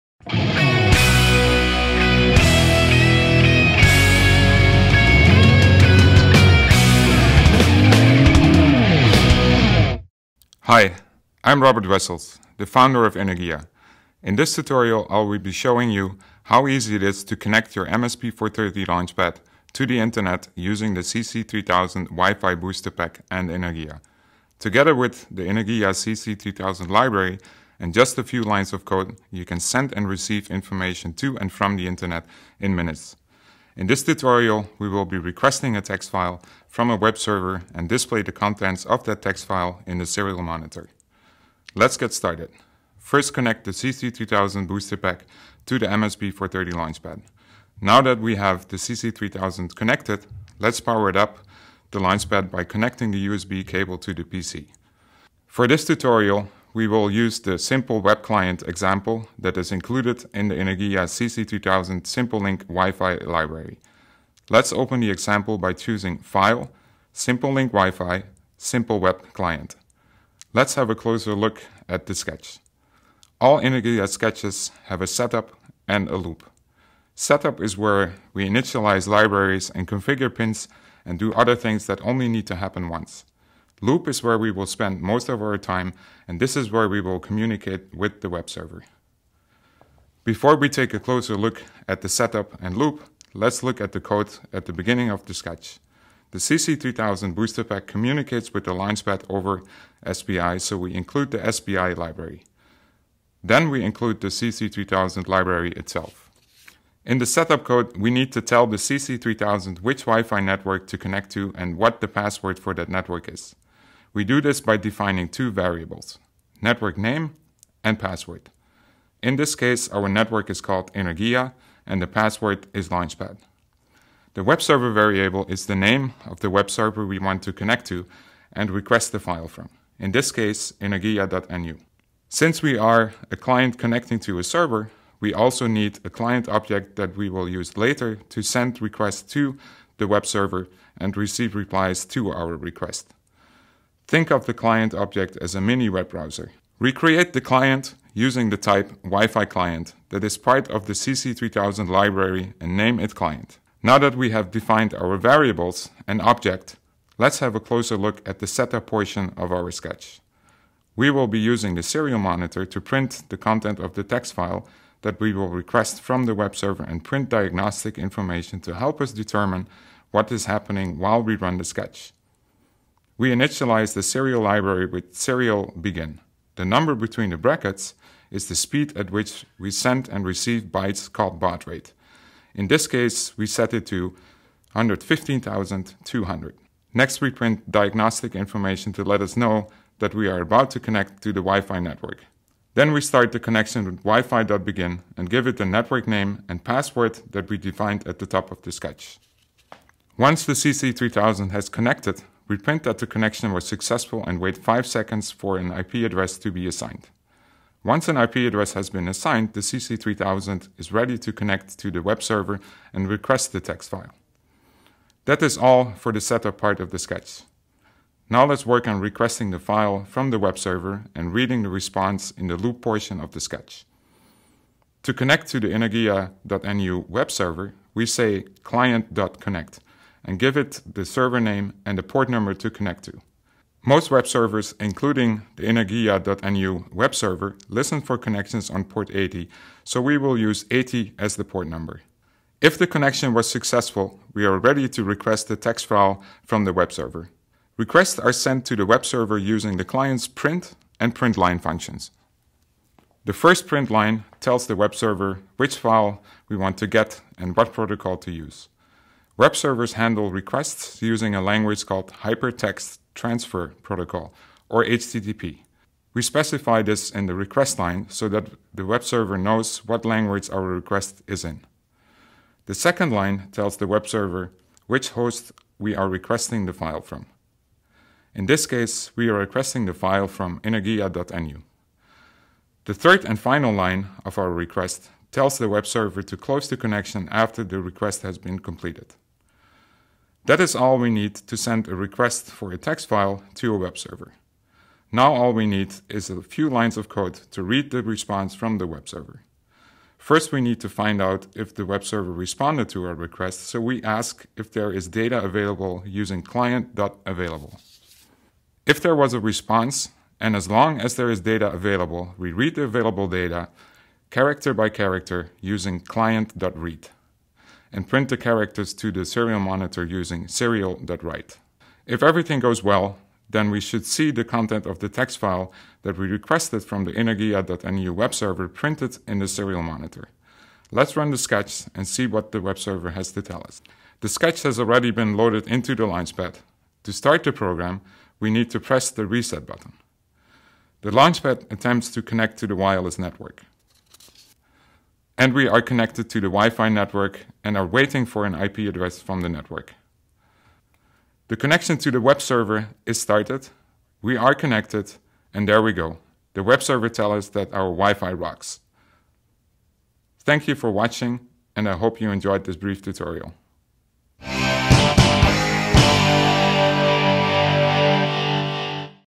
Energia Tutorial: CC3000 Web Client
This video tutorial shows you how to connect to the internet using the CC3000 WiFi BoosterPack.

NOTE: CC3000 is no longer supported in Energia. It is recommended you use CC3100 which is a substantial upgrade in Wi-Fi performance.